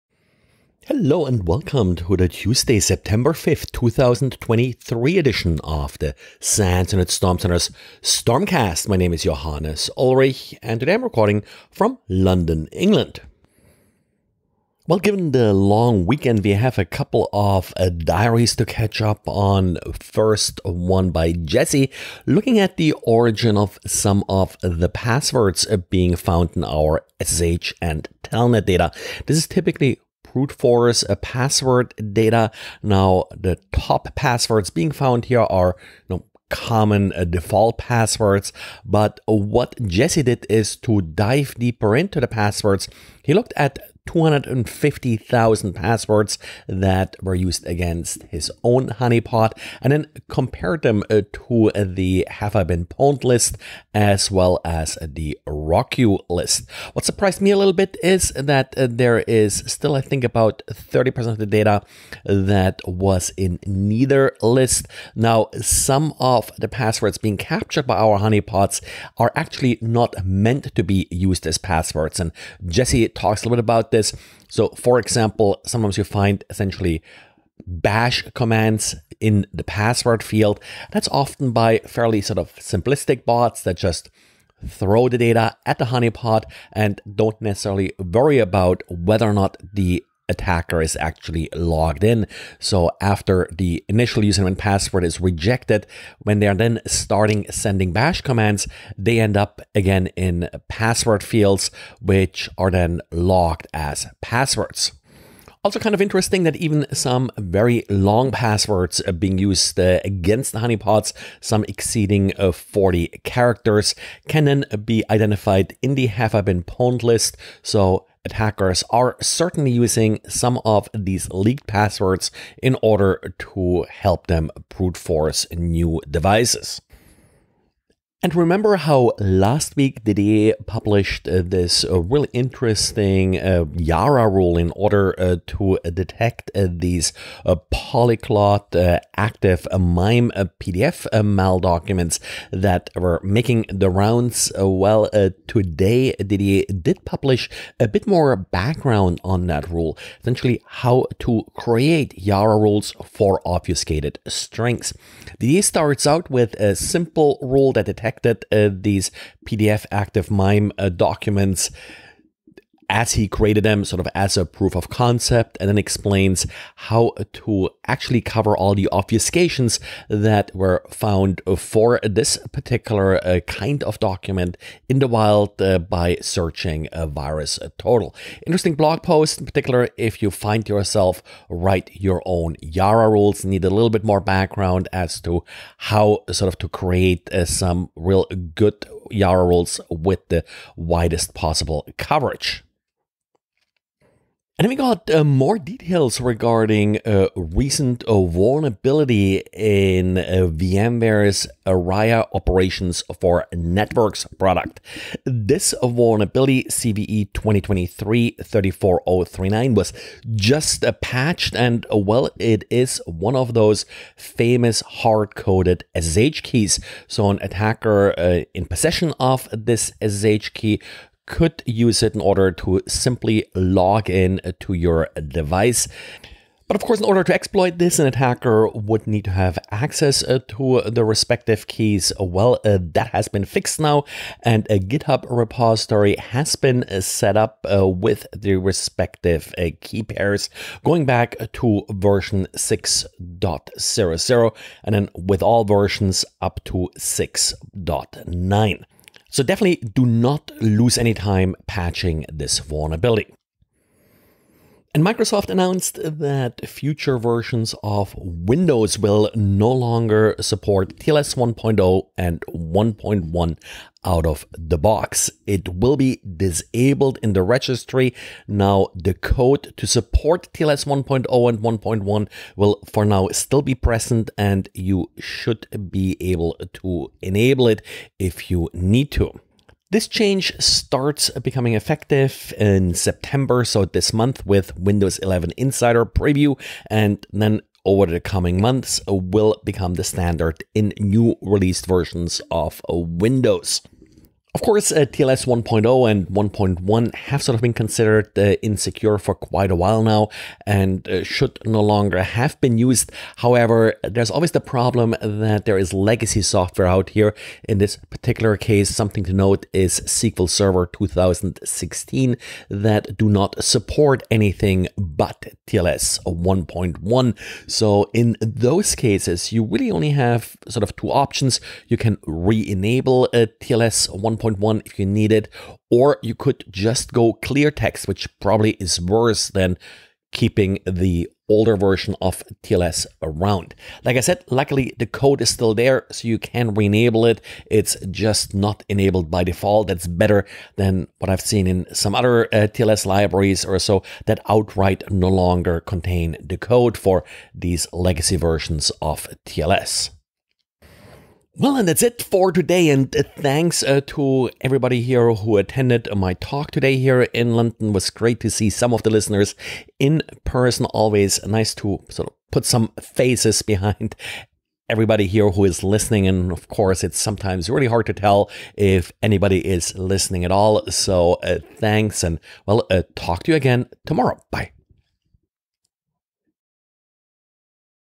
Network Security News Summary for Tuesday September 5th, 2023
Password Origins; YARA Rules for Obfuscated Strings; VMware Aria Keys; Windows TLS 1.0/1.1;

What is the Origin of Passwords Submitted to Honeypots